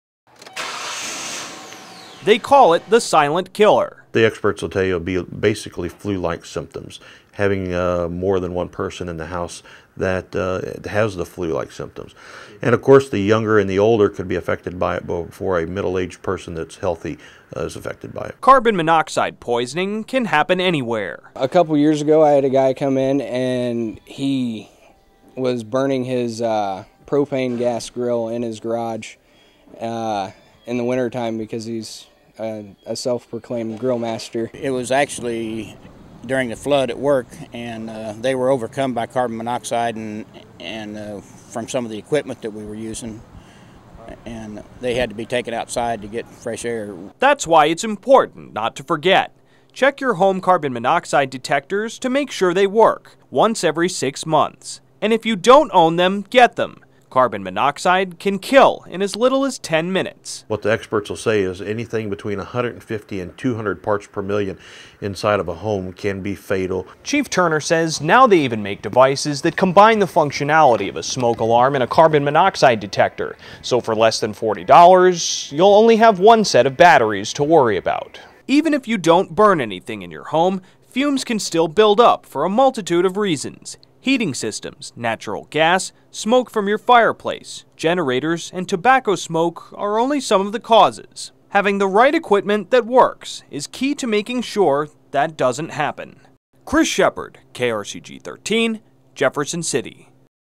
Check your home for "the silent killer"
Checking and changing batteries in your home's smoke detectors isn't the only thing you may need to do in order to stay safe. KRCG13's Chris Sheppard reports.